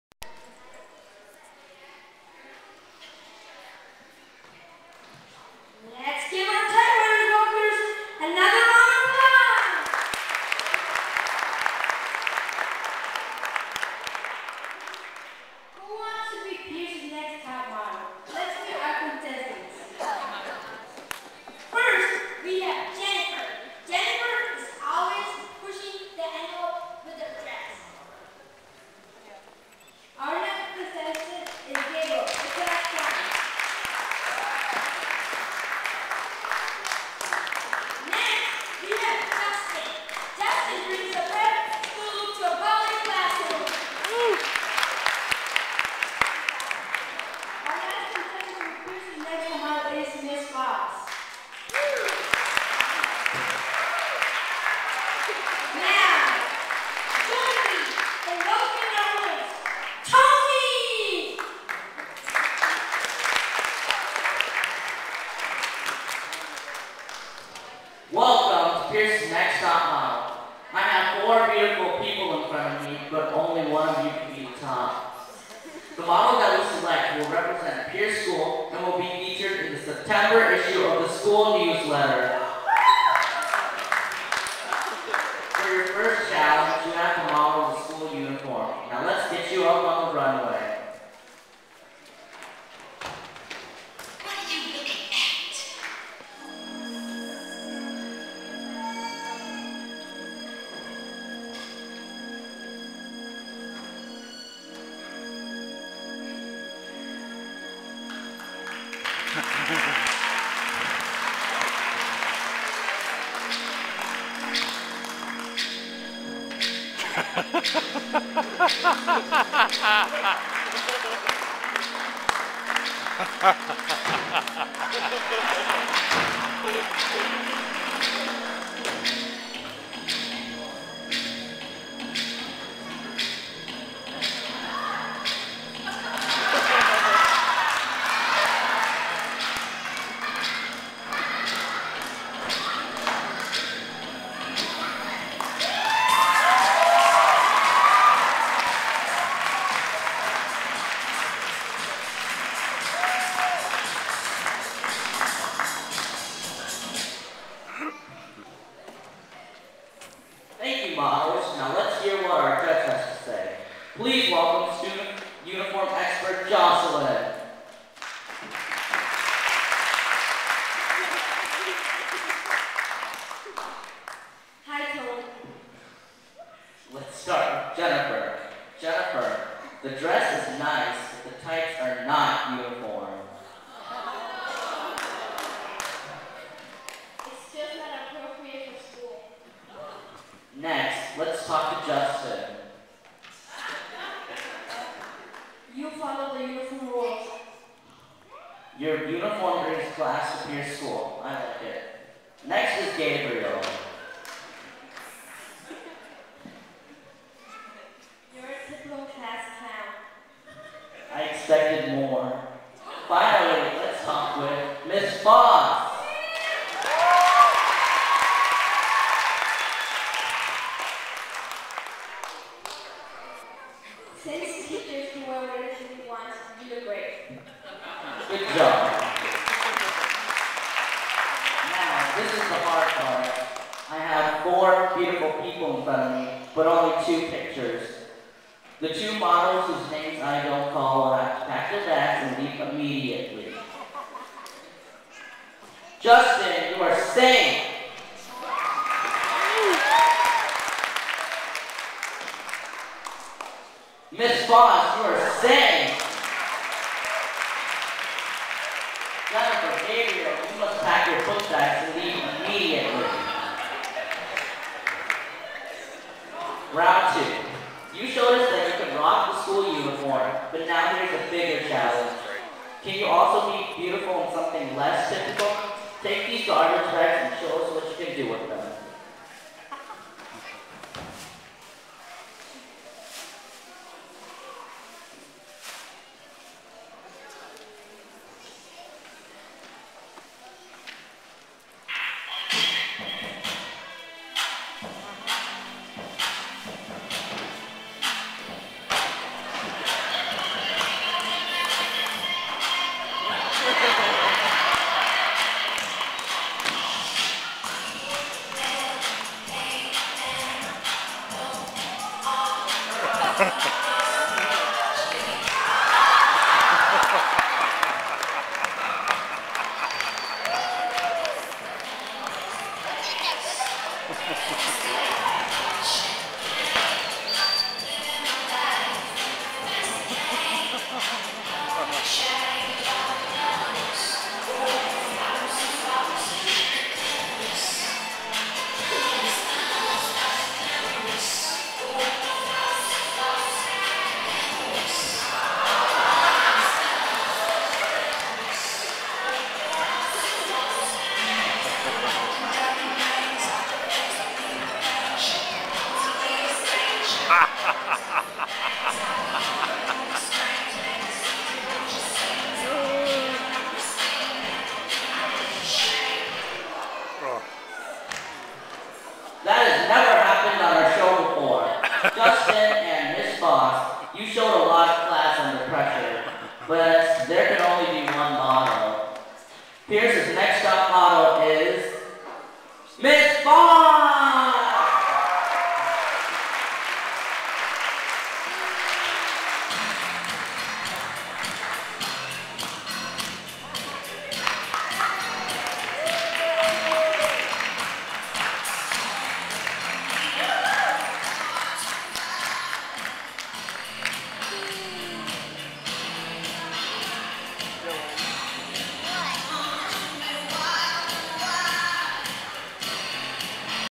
Peirce's Next Top Model, Peirce Elementary School Circus, 2010
For the last three years, Peirce has hosted a fabulous circus.  This is from the evening performance.